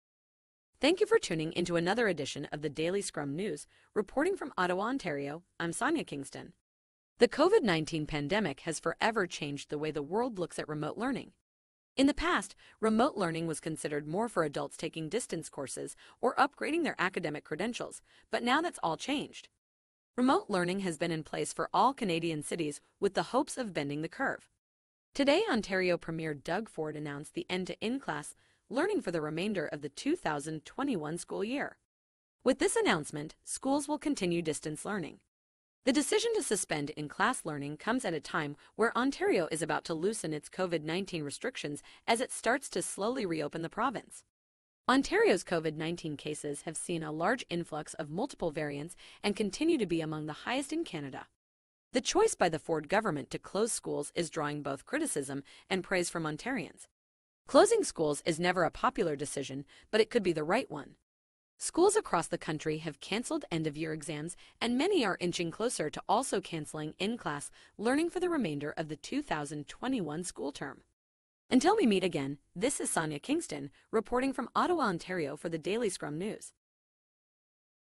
Ontario Suspends In-Class Learning Indefinitely
The COVID-19 pandemic has forever changed the way the world looks at remote learning. In the past, remote learning was considered more for adults taking distance courses or upgrading their academic credentials but now that's all changed.